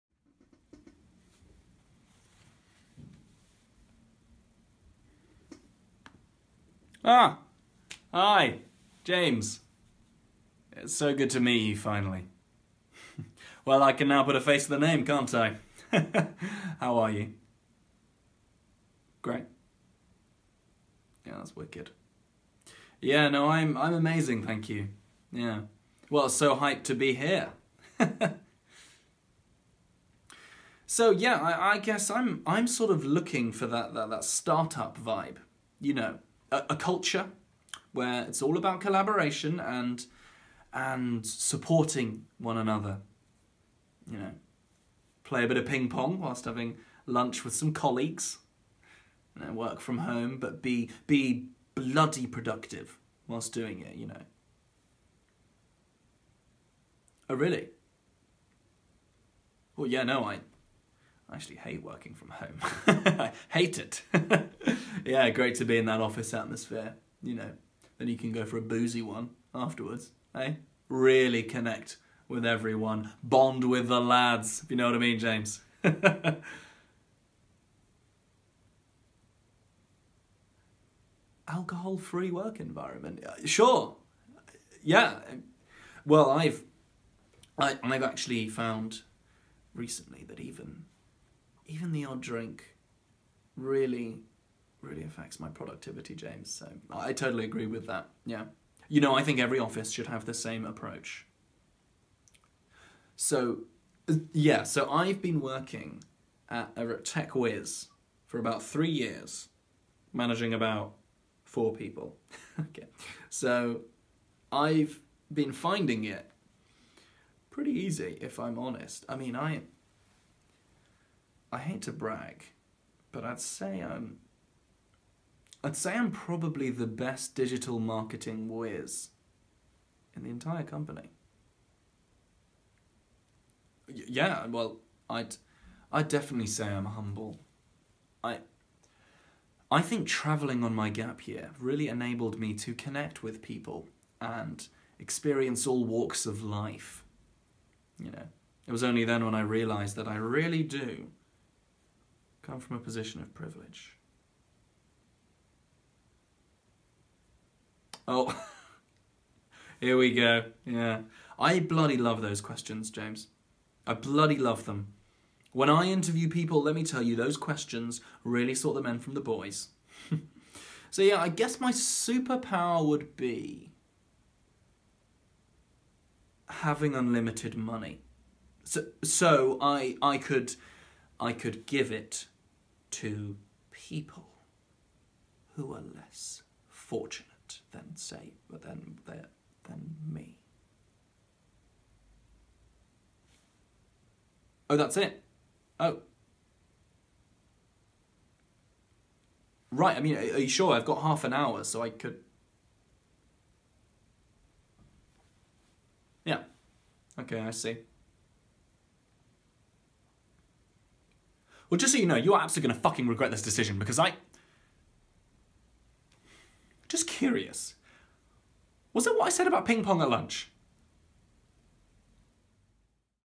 Job Interviews During Covid (Cheeky Monologue)
This is a little monologue written by the brilliant Sophie Walton.

I think it's funny...

Xx